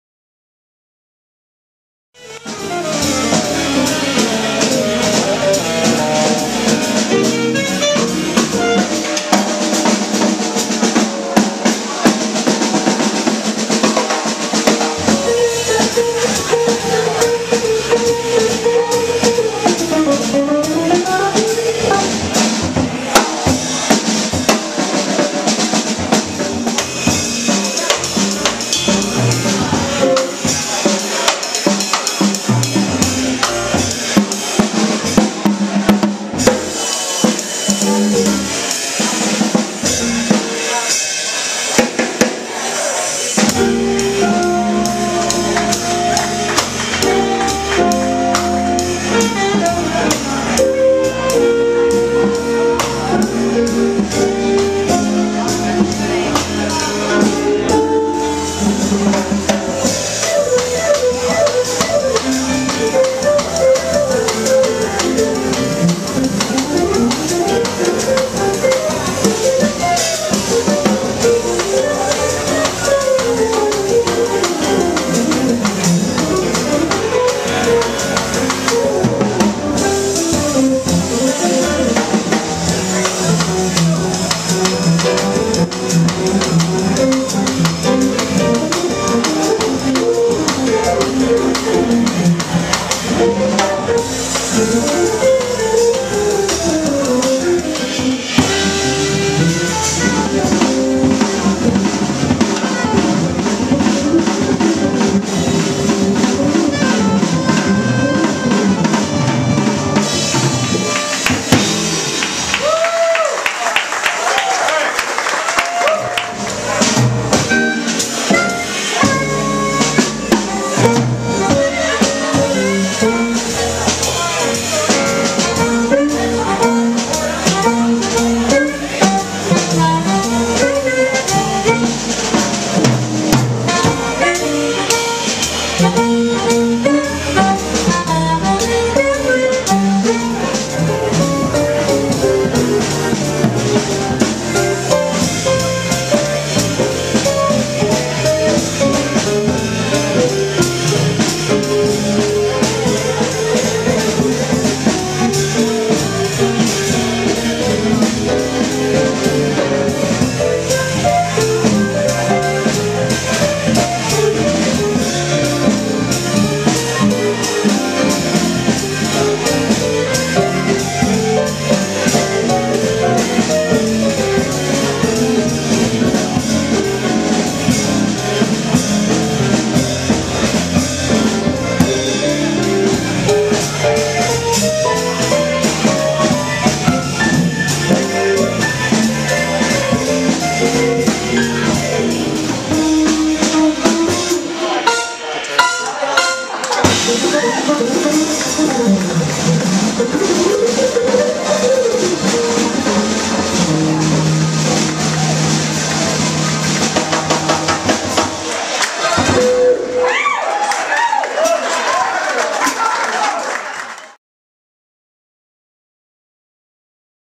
Randissimo's Sunday Jazz at SpeakEZ Lounge, Grand Rapids, MI, 10/21/12
Story at localspins.com: Drummer Randy Marsh, guitarist Carlos Melendez, pianist Mark Kahny and saxophonist Tony Scott perform during the debut of Sunday night jazz at Grand Rapids' SpeakEZ Lounge on Oct. 21, 2012.